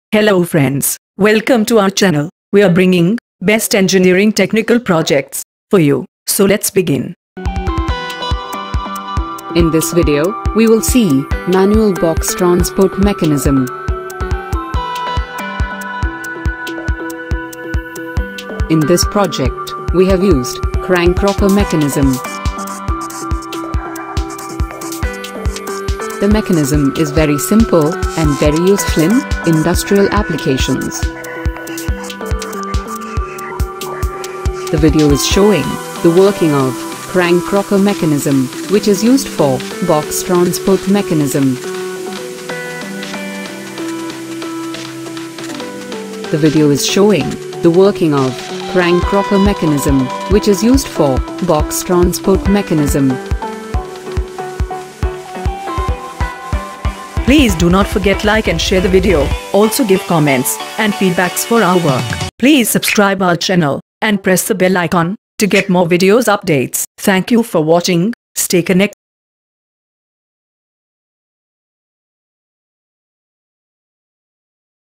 MANUAL BOX TRANSPORT MECHANISM | BOX SHIFTING MECHANISM | THOMSON MECHANISM | SMART CONVEYOR
HELLO FRIENDS,
In this project, we have focused on to develop simple mechanism / machine in which box type or stamping type material, we can move neatly and hence we can improve efficiency.